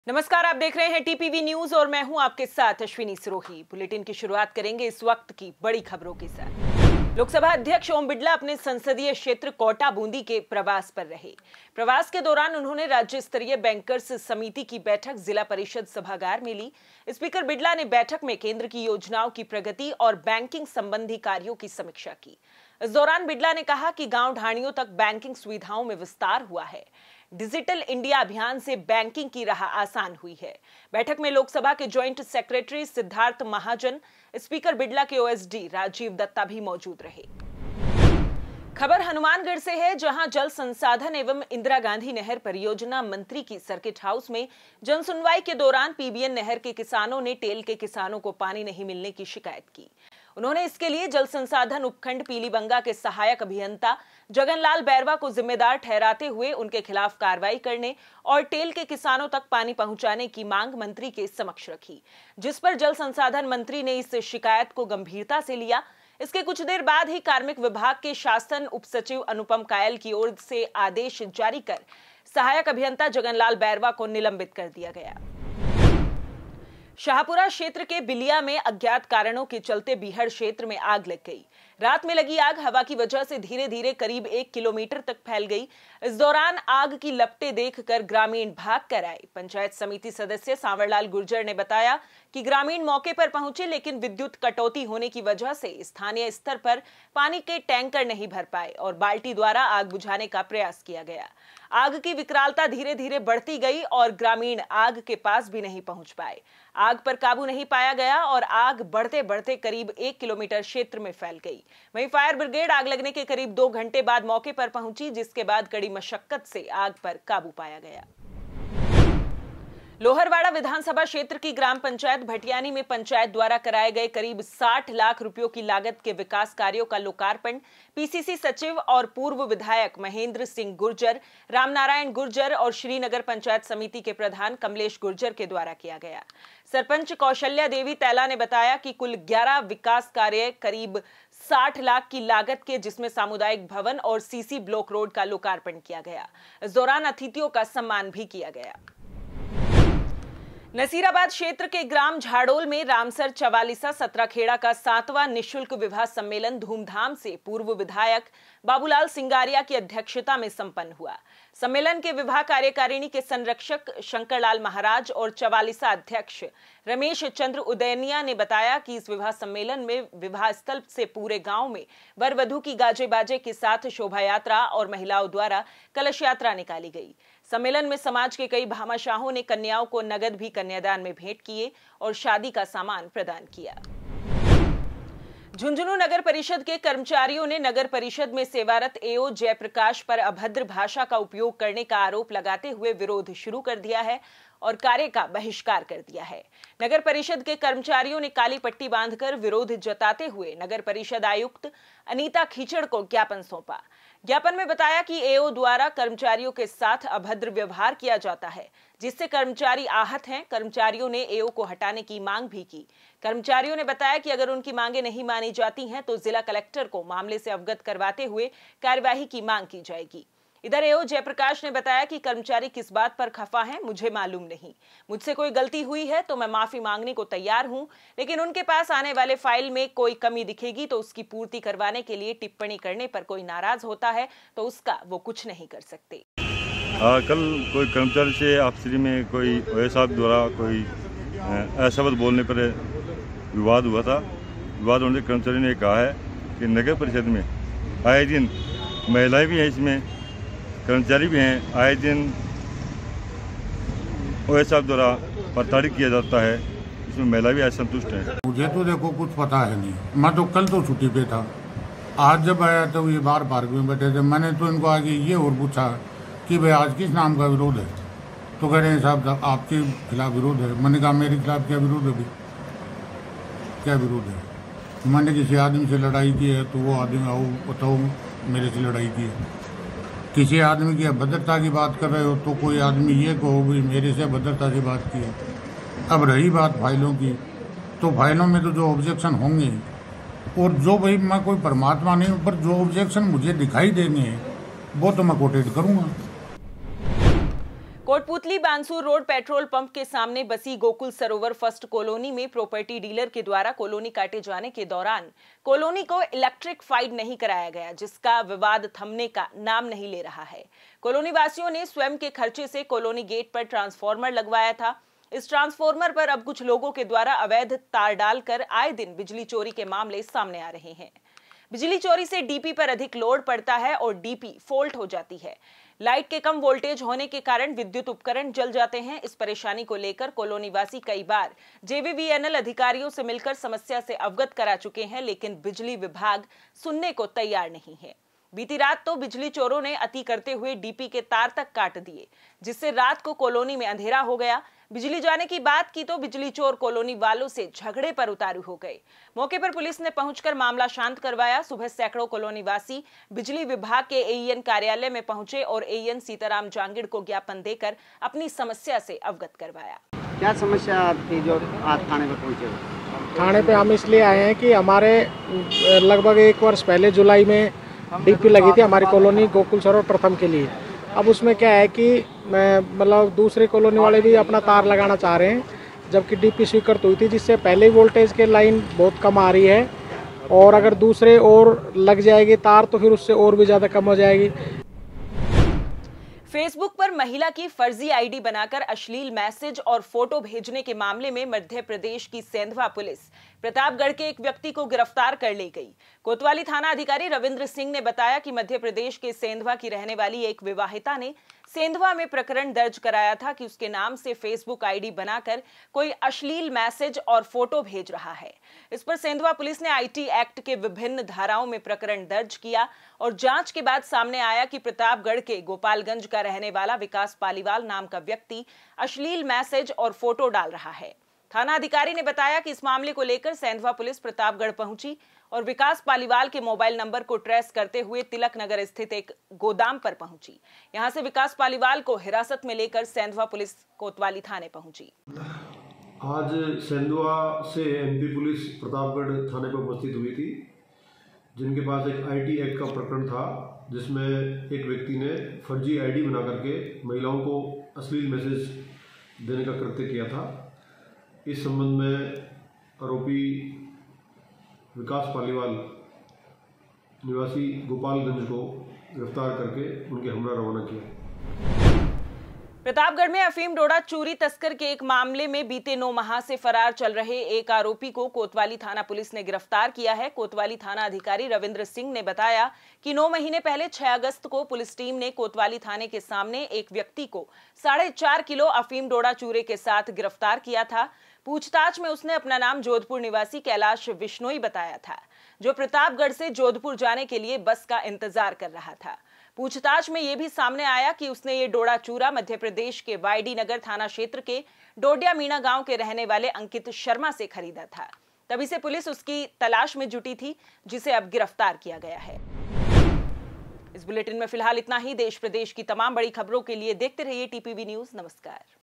Rajasthan Water Crisis | परेशान कॉलोनी वालो ने बोला पावर हाउस पर धावा | TPV NEWS
1 स्पीकर बिरला अपने संसदीय क्षेत्र के प्रवास पर - जिला परिषद सभागार में अधिकारियों की ली बैठक
2 टेल के किसानों को पानी नहीं मिलने का मामला - जल संसाधन मंत्री से शिकायत के बाद एईएन निलंबित
3 बिलिया मे बीहड़ क्षेत्र में आग लगी- कड़ी मशक्कत के बाद पाया आग पर काबू
4 भटियानी पंचायत में हुए विकास कार्यों का हुआ लोकार्पण- 60 लाख की लागत से कराए गए विकास कार्य
5 लोहरवाड़ा रेगर समाज का नि:शुल्क सम्मेलन सम्पन्न- 13 नवविवाहित जोड़े परिणय सूत्र मेंबंधे
6 झुंझुनूं नगर परिषद झुंझुनूं के एओ का विरोध-  एओ के विरोध में कर्मचारियों ने किया विरोध प्रदर्शन
7 परेशान कॉलोनी वालो ने बोला पावर हाउस पर धावा- एईएन को ज्ञापन सौंप कार्रवाई की रखी मांग
8 फर्जी आईडी बनाकर अश्लील मैसेज भेजने का मामला - पुलिस ने आरोपी को किया गिरफ्तार
9 माह से फरार आरोपी गिरफ्तार - अफीम डोडा चूरे के मामले में था आरोपी फरार